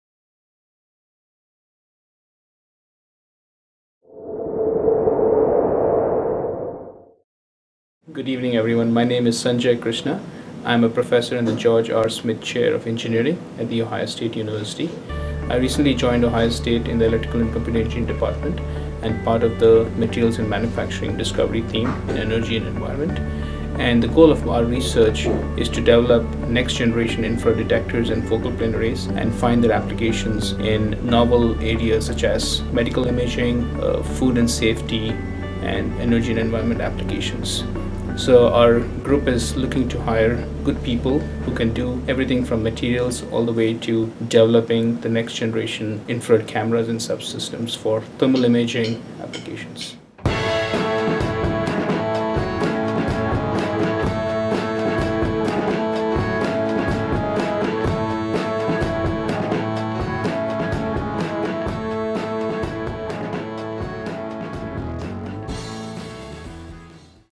Ohio State University Discovery Themes: Dr. Sanjay Krishna, ECE
The Department of Electrical and Computer Engineering (ECE) at The Ohio State University welcomed its newest faculty member in January, the first in support of the university's ongoing Discovery Themes Initiative.
Dr. Sanjay Krishna joined ECE as professor and George R. Smith Chair in Engineering, assisting Ohio State’s Materials and Manufacturing for Sustainability (M&MS) Discovery Theme led by Professor Steven Ringel. The M&MS Discovery Theme is an effort at Ohio State to create pre-eminence in materials and technologies for sustainability, focusing on innovation and industry deployment. Krishna is a world-leading researcher and innovator in the field of narrow bandgap semiconductors applied to infrared imaging sensors and related technologies.

Video by: Ryan Horns, Ohio State ECE PR Coordinator